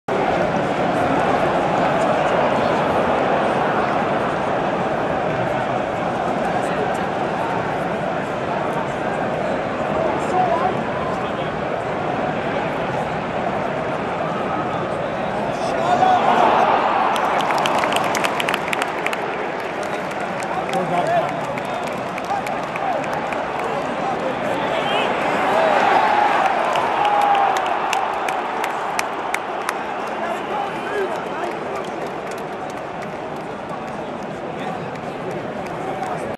Kante sneaky tackle from Luis Dias
Chelsea vs Liverpool
27 February 22
Wembley Stadium